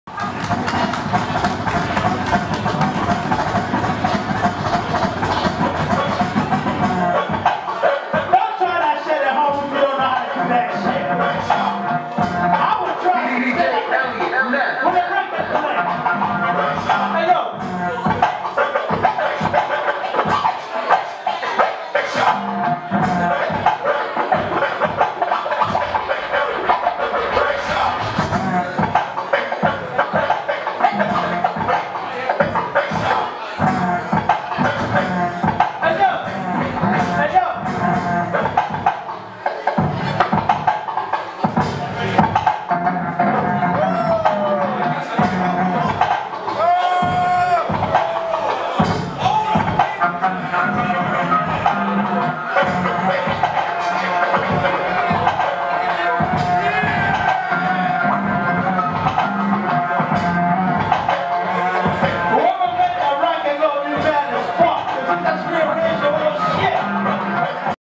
DJ Elliot Ness Flipping A Beat Live w/ Dj G7
Wreck Shop Movement Presents: The Formula {Episode 1} Preview